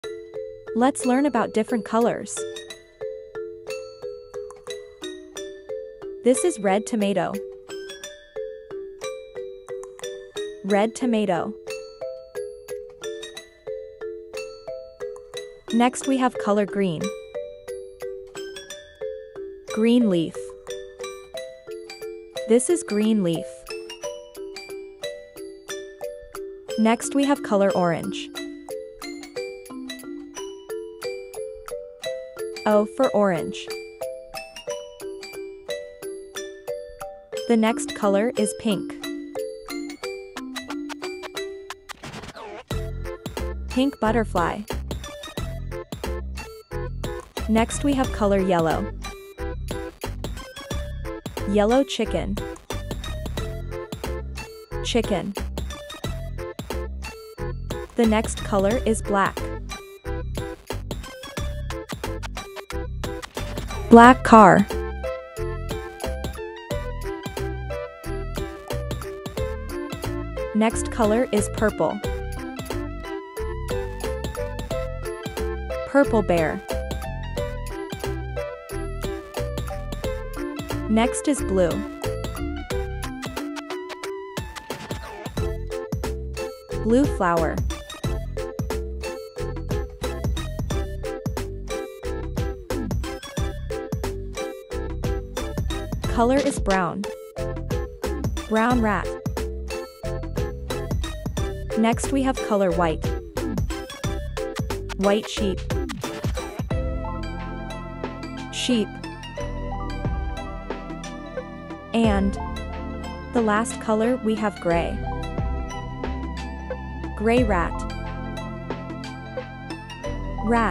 learn colors / color names
Welcome to our fun and colorful adventure!
In this video, kids will learn about different colors through exciting animations, catchy songs, and playful characters. Perfect for toddlers and preschoolers, this video helps children identify and name colors like red, blue, yellow, green, and more. Join us as we explore the vibrant world of colors through everyday objects, animals, and nature!

✨ Great for early learning and development 🎵 Fun songs to sing along with 🖍️ Perfect for ages 2-5

Transcript